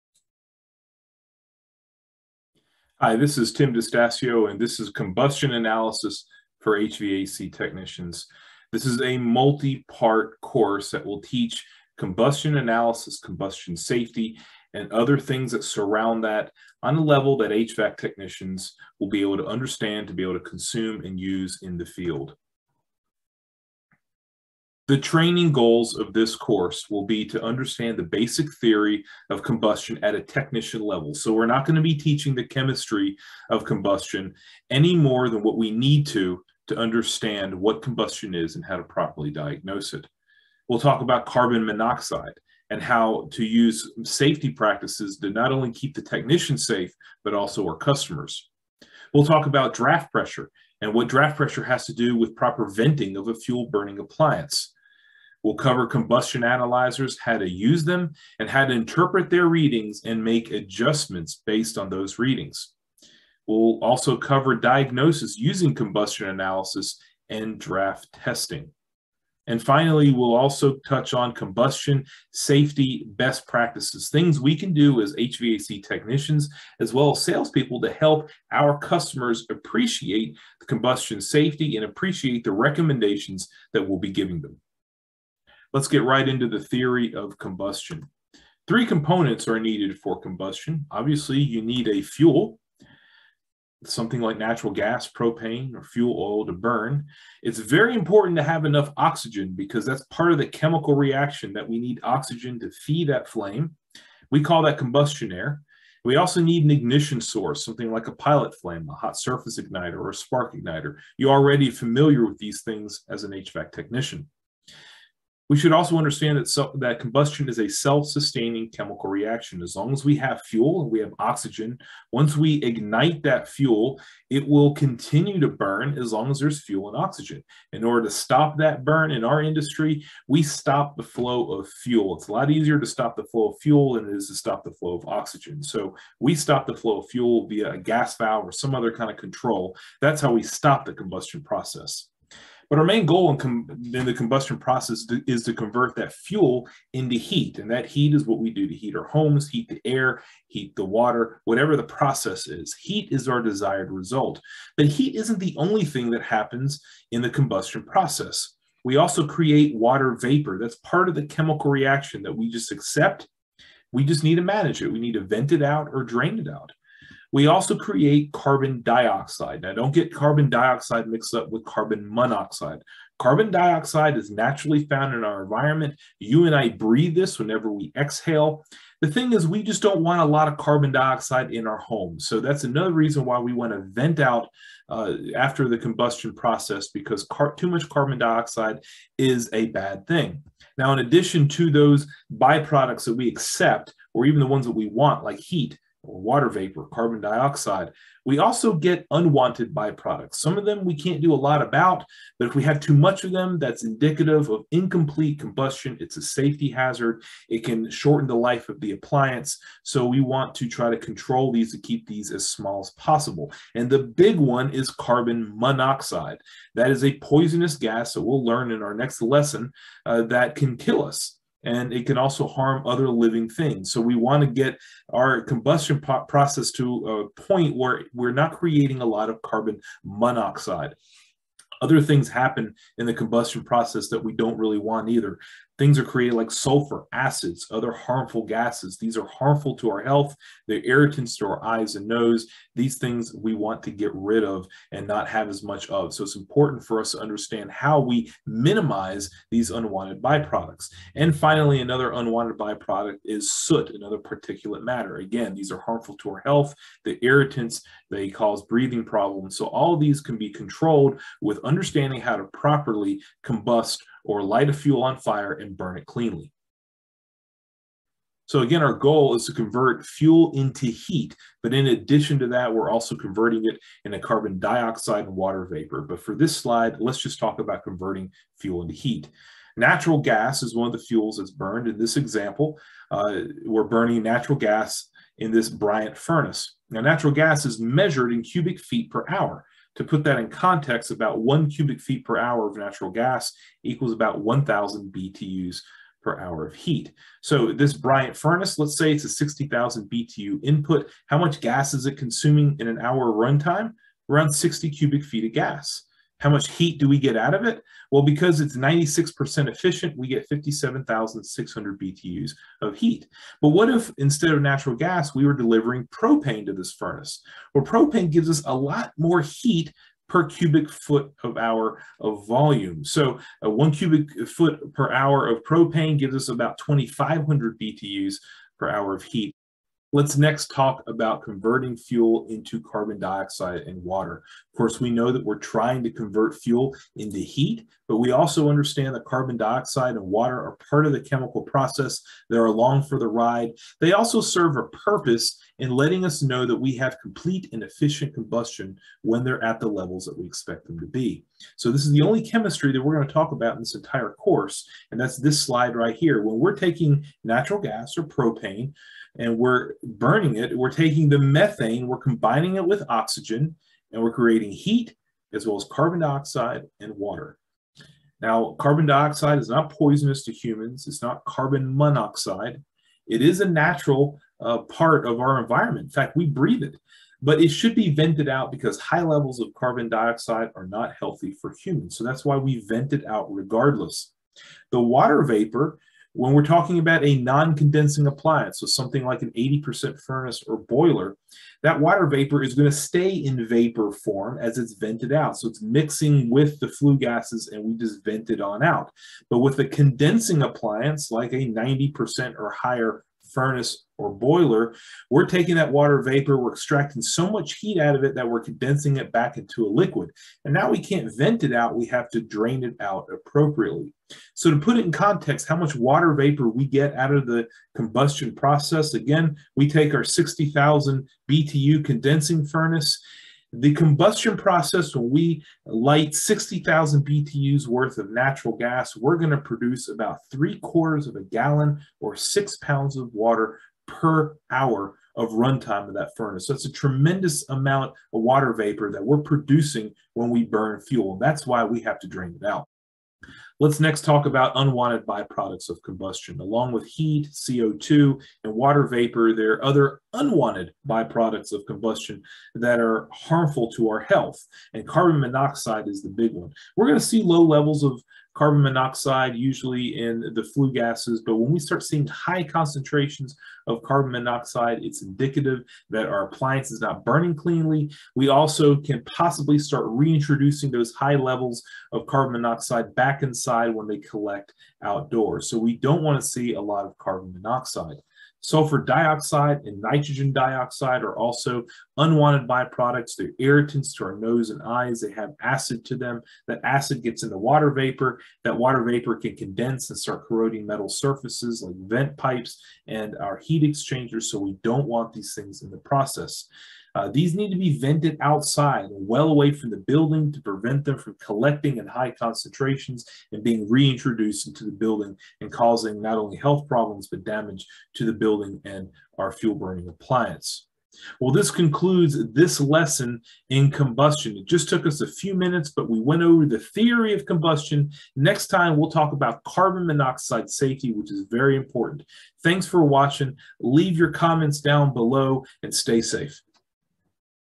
Combustion Analysis Part 1
In this video, we introduce the concepts of combustion analysis for HVAC technicians and talk about the wanted and the unwanted by-products of combustion.